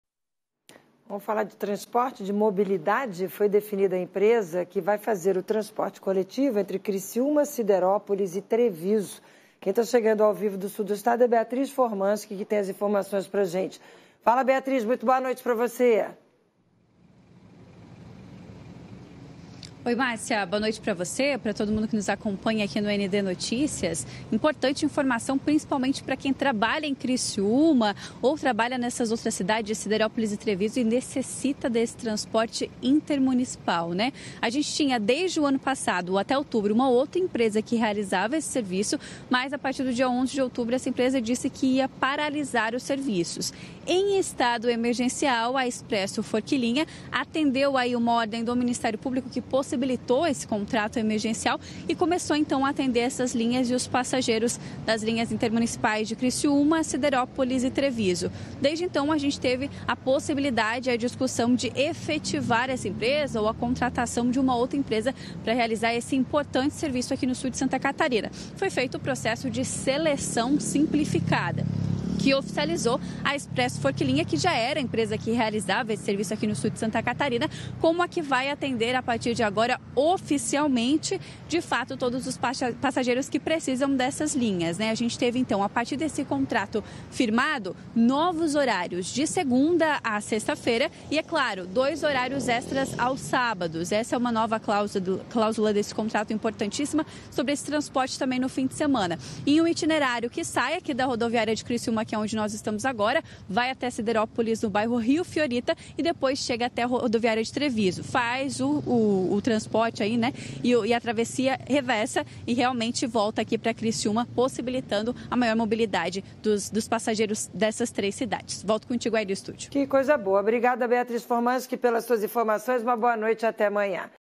Nova parceria: Expresso Coletivo Forquilhinha operará em Criciúma, Siderópolis e Treviso
A empresa Expresso Coletivo Forquilhinha foi selecionada para operar o transporte coletivo entre as cidades de Criciúma, Siderópolis e Treviso. Após o processo de licitação, a escolha foi feita visando melhorias nos serviços de transporte de passageiros nessas localidades. Essa decisão representa uma nova fase para o transporte público na região, com a expectativa de oferecer maior qualidade e eficiência para os usuários. Confira.